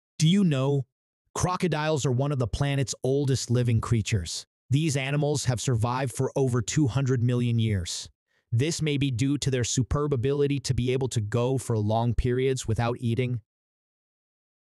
Crocodiles: Among the Oldest Living Creatures on Earth
What makes crocodiles one of the oldest living creatures on the planet?

What is the secret behind crocodiles' ability to survive for over 200 million years?

How long can crocodiles go without eating?